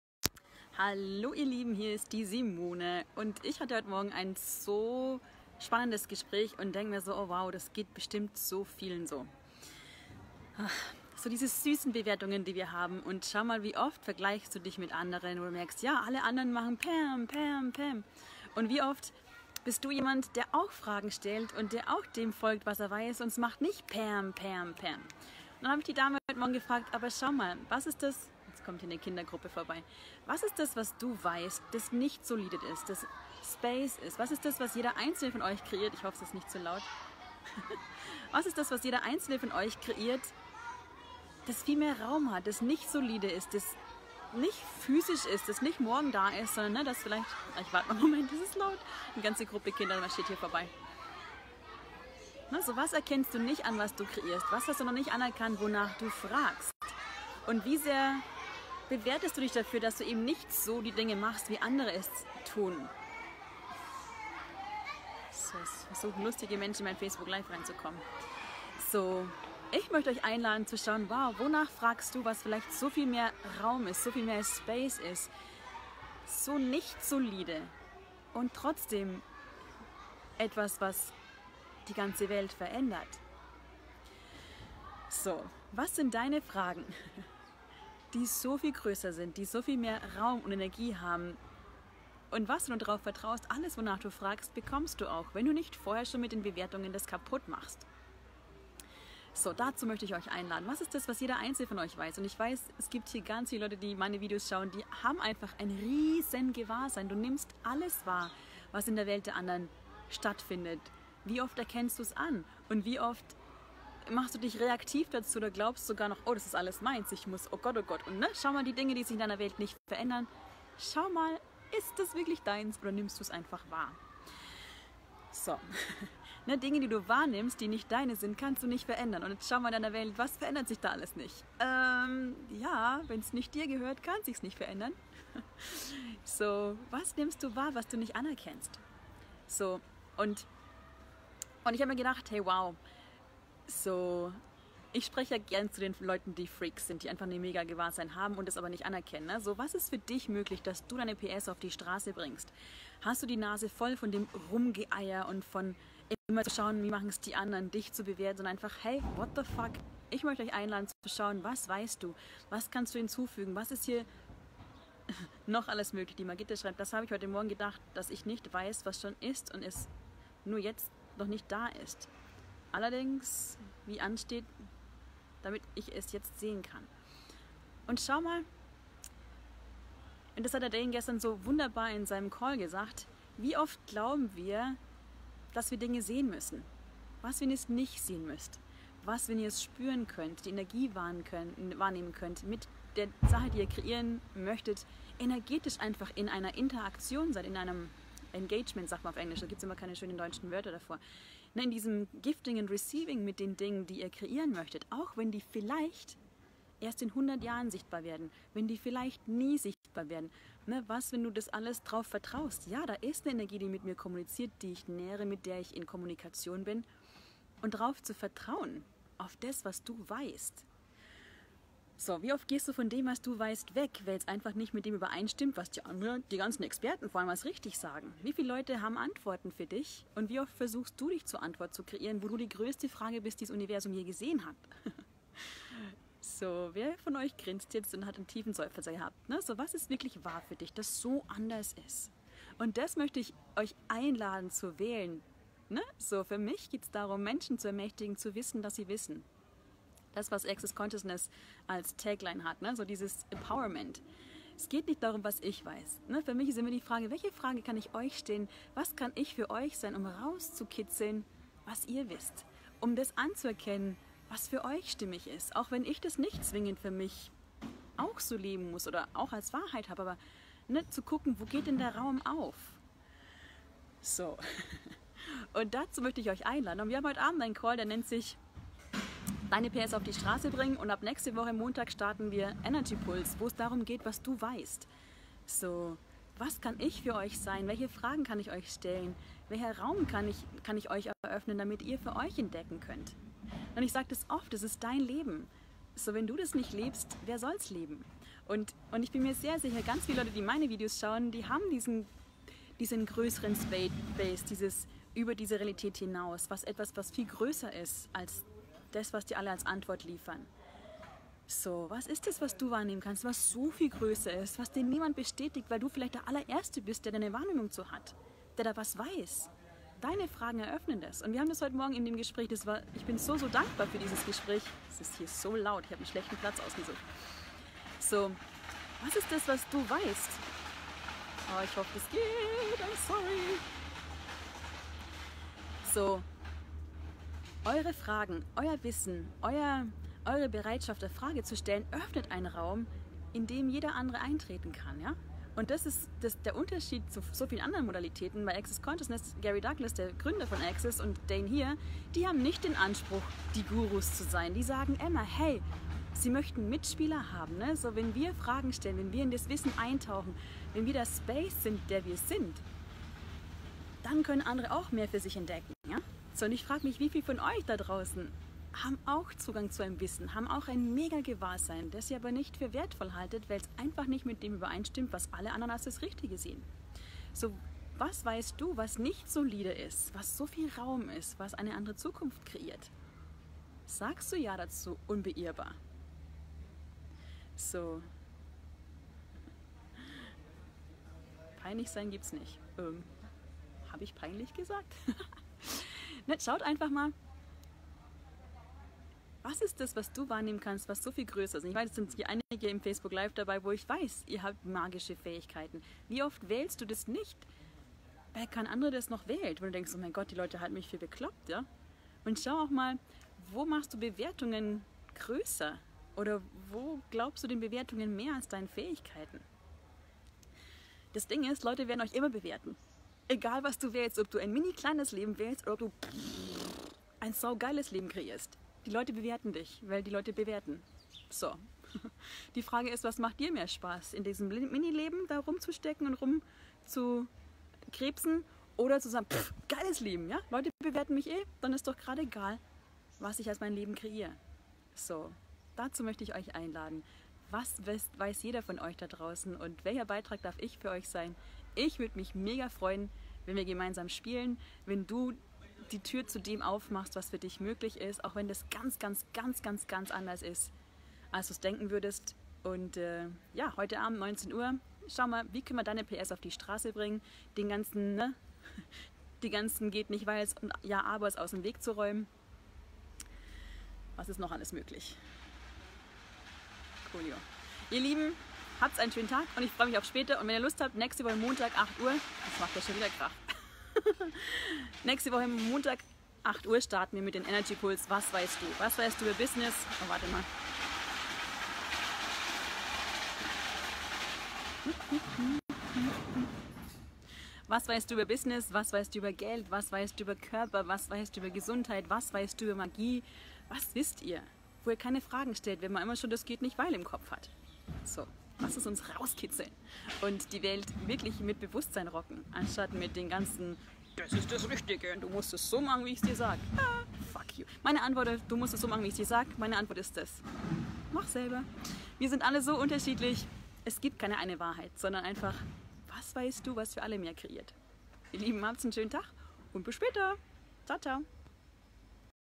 Bewertungen und was ist es das du wahrnimmst, das so anders ist
Wie oft bewertest du dich, dass es bei allen anderen zu funktionieren scheint nur bei dir nicht?
Doch hast du schon mal anerkannt, was es ist, wonach du fragst?
Ist das vielleicht sehr viel größer und sehr viel undefinierter als die meisten gewillt sind zu haben?
Welcher Beitrag darf ich für dich sein all das für DICH zu entdecken und mehr und mehr dem zu vertrauen, das du WEISST.

heute Abend 19 Uhr bringen wir DEINE PS auf die Strasse
und nächste Woche 16.09. um 8 Uhr für 40 Tage
Was weisst du?